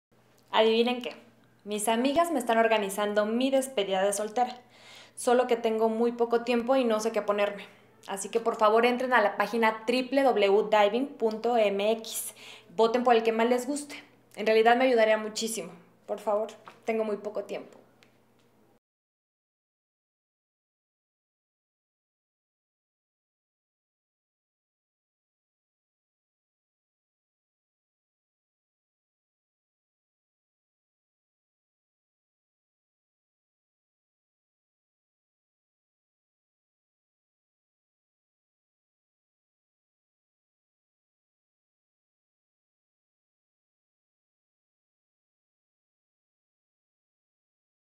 Despedida de soltera. ¡Auxilio!
Luz Casillas no sabe qué ponerse para su despedida de soltera. ¡Ayúdala con tu voto!
¿Quieres ASISTIR a la Despedida de Soltera? y averigua cómo.
¡PROXIMAMENTE!
Una historia transformada en experiencia.
Creado y Dirigido por Benjamín Villeda (@benjaminvilleda)
Feat. Luz Casillas (@luzecasillas) & Juan Pablo Campa (@juanpablocampa)
Prod. Gigantic Films (giganticfilms) & MediaBrain (@mediabrainmx)
Participación especial de:
Florencia Ríos (@perrarios)
Gustavo Monroy
Jose Luís Allier